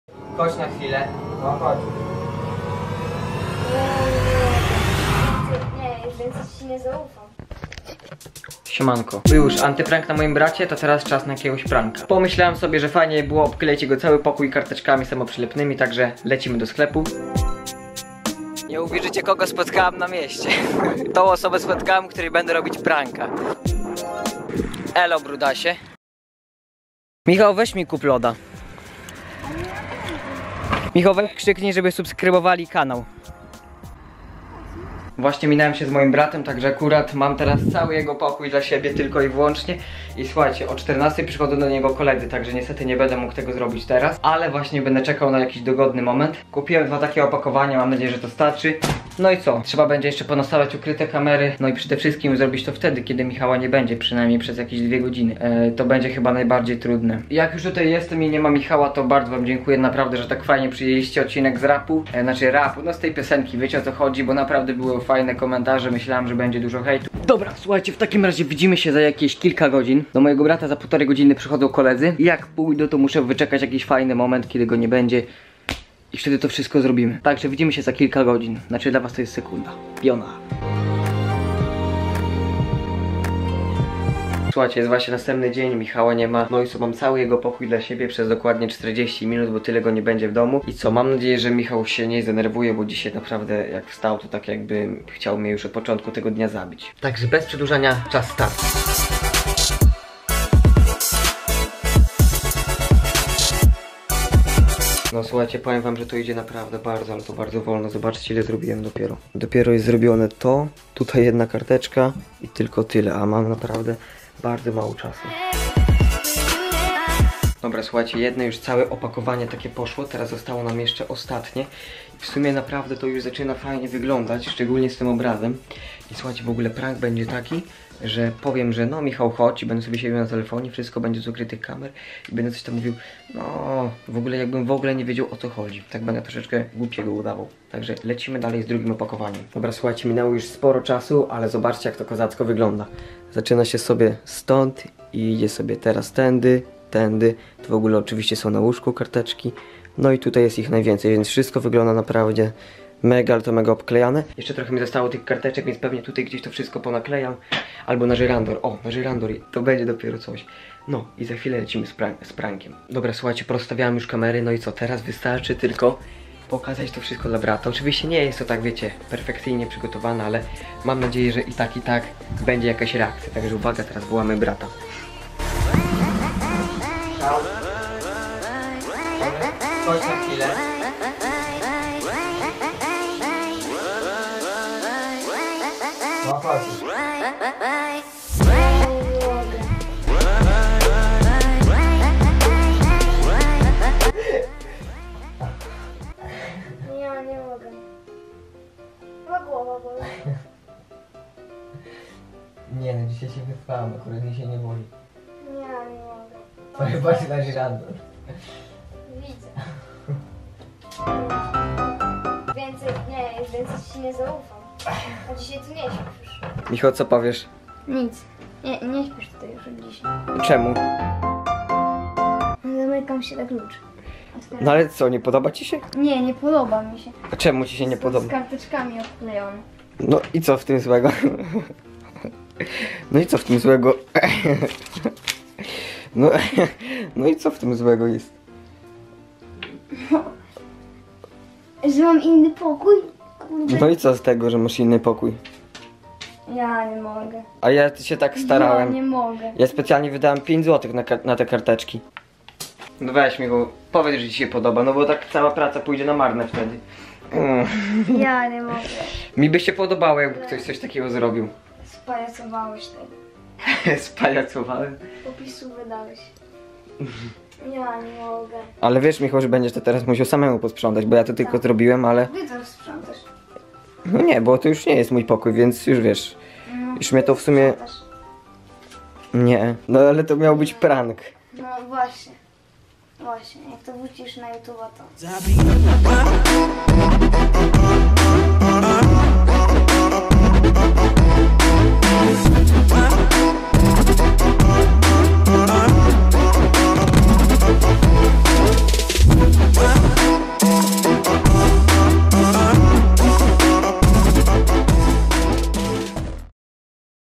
CAŁY POKÓJ W KARTKACH?! |Prank na bracie
Siemanko, oto prank na moim bracie ;D
W sumie sami zobaczcie co i jak.